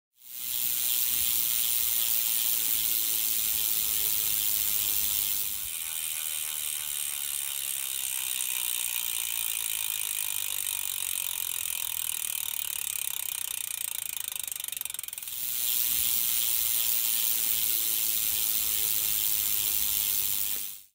Hub Sound - Hope Pro 2 EVO (Mountain Biking) (4K)
Ever wondered what the Hope Pro 2 EVO rear hub sounds like? Wonder no more...